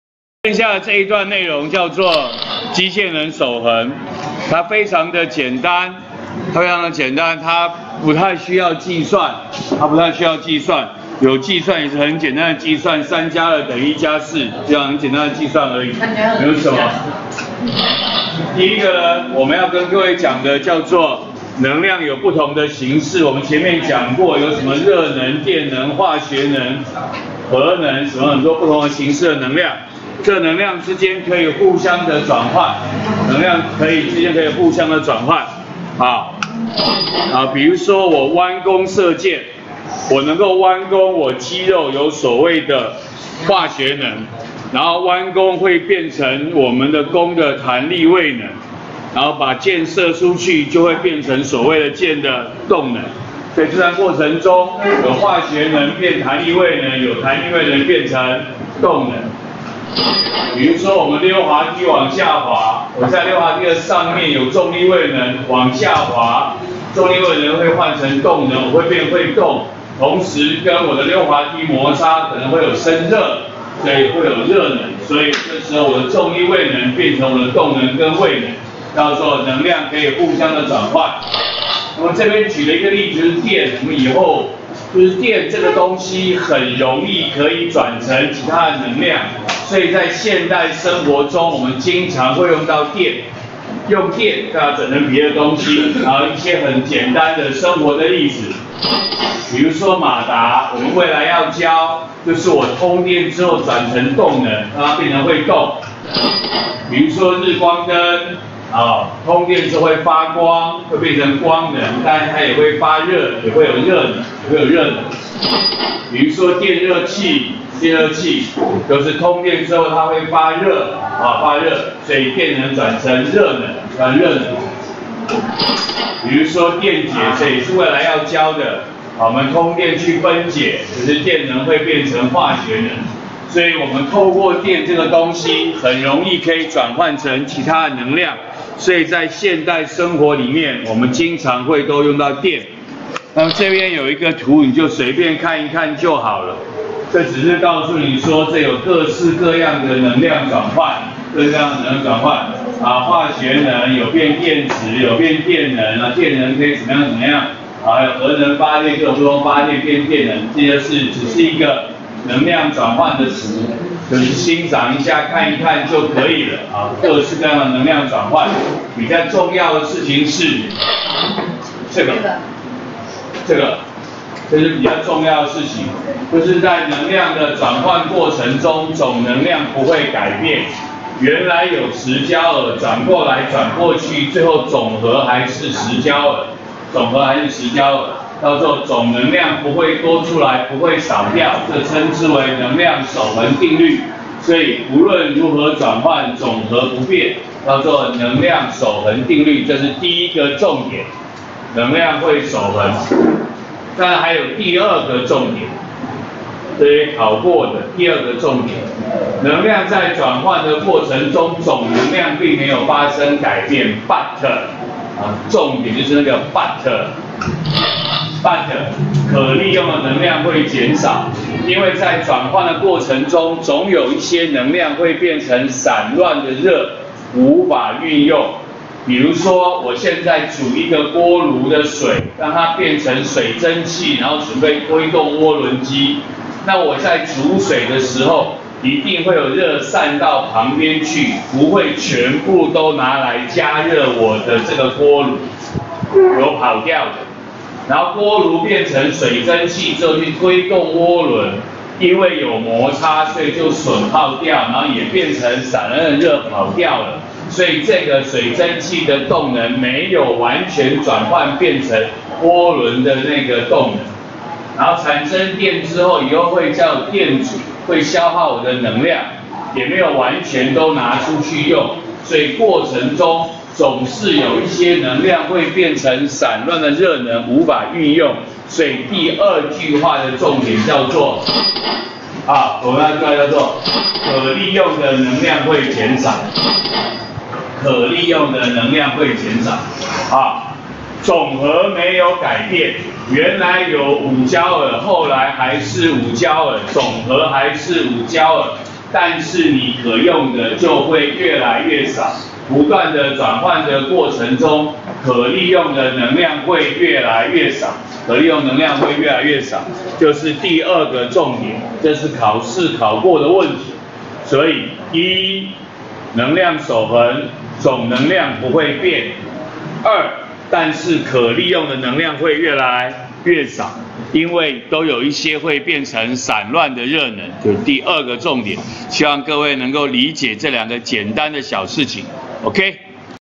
106學年國三理化＿功與能16：能量轉換；能量守恆；可利用的能
說明能量有不同形式，可以互相轉換，指出電能轉換成其它能量。說明能量守恆定律，及可利用的能量會減少。